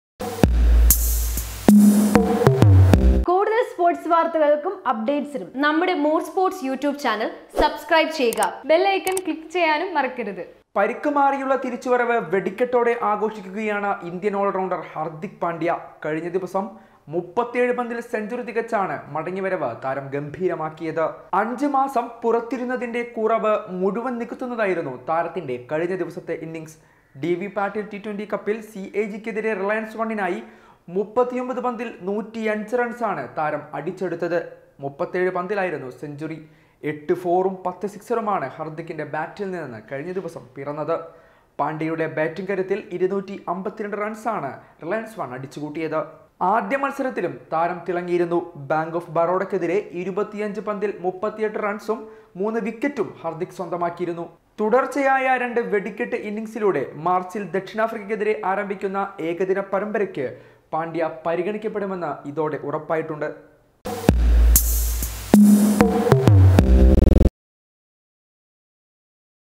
ഹർദിക് പാണ്ട്യയുടെ വെടിക്കെട്ടു തിരിച്ചു വരവ് - 39 പന്തിൽ 105 !! - PANDYA MASS !!
More Sports Analysis,